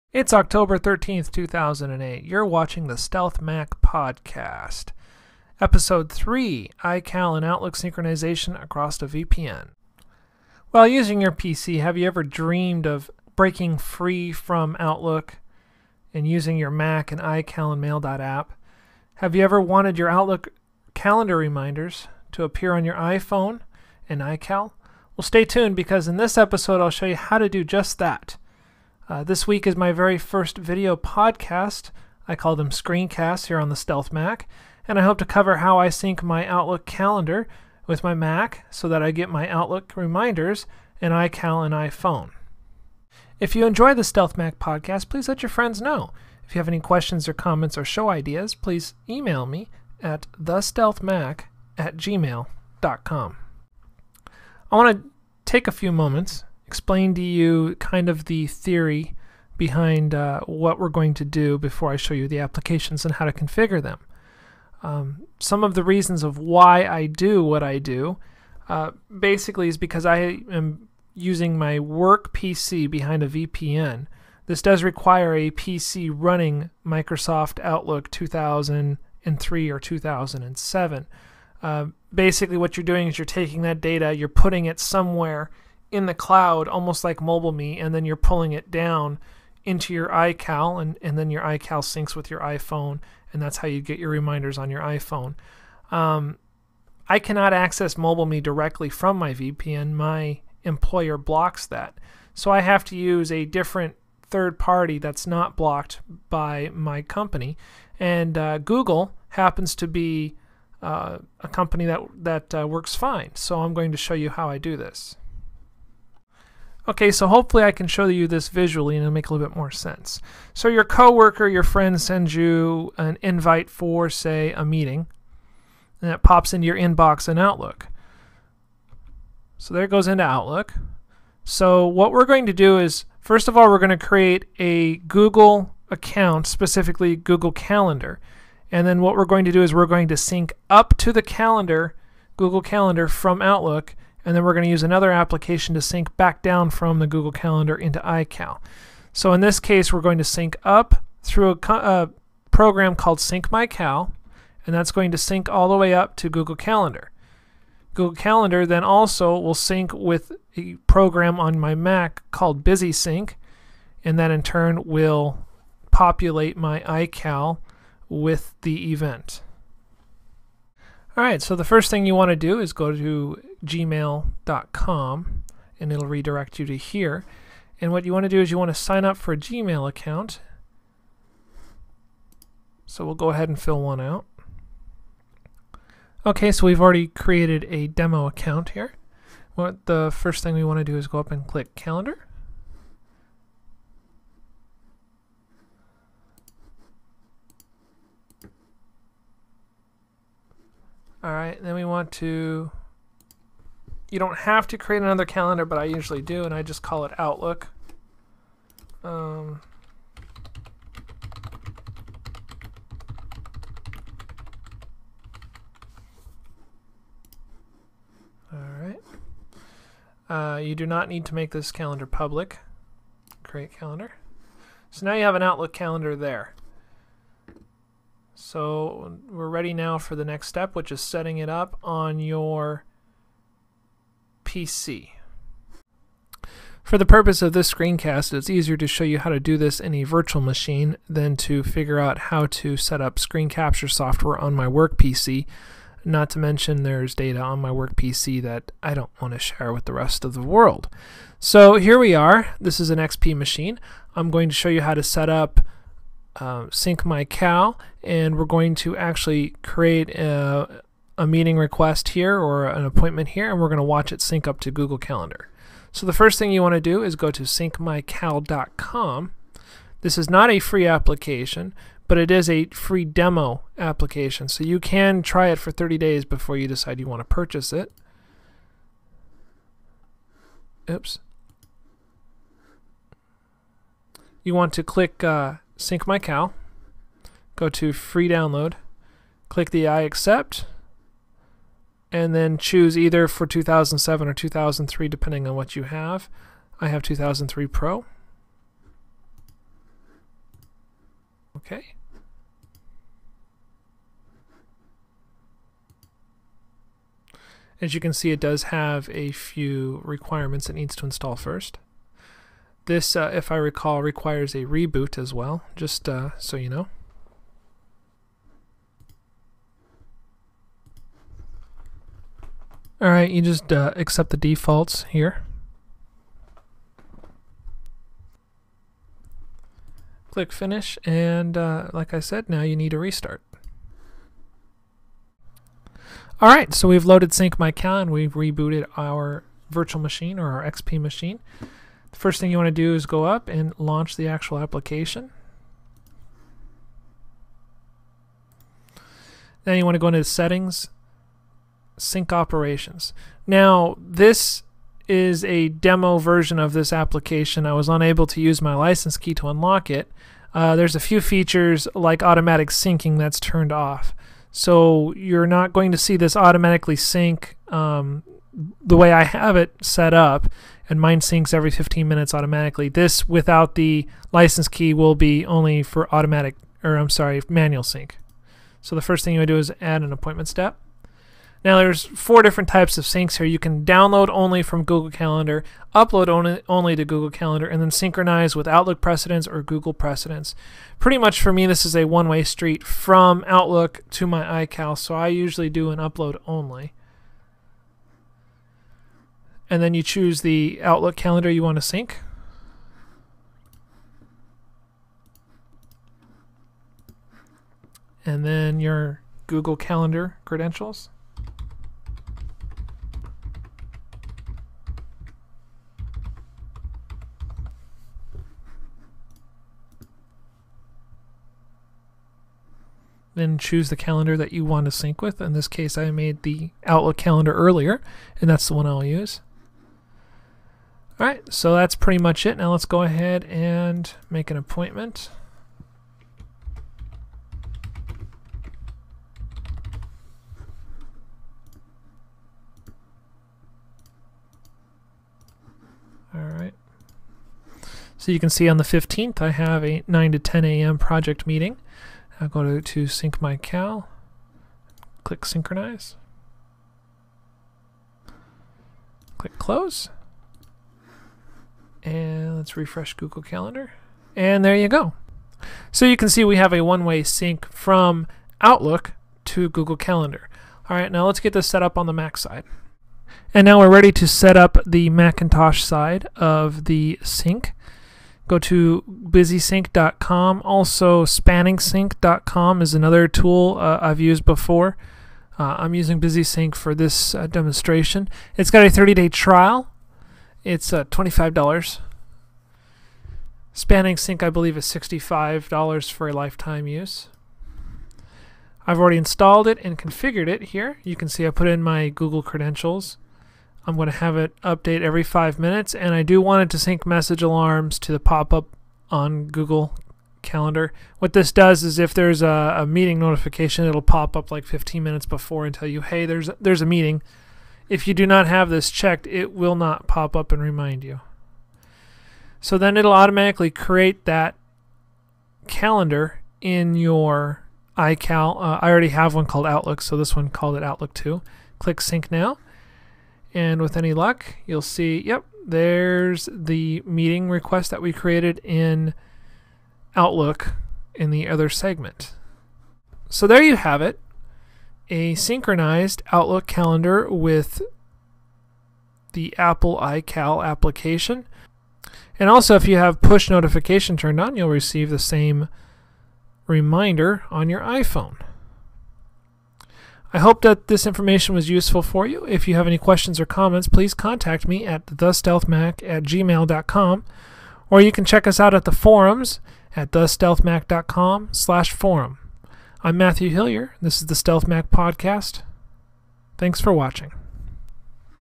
Episode 003 - Synchronizing your iCal and Exchange across a VPN
In this episode I show you how I sync my work outlook calendar with my iCal using "cloud" technology. Since I cannot hit mobile me directly from my office PC I had to get a little creative.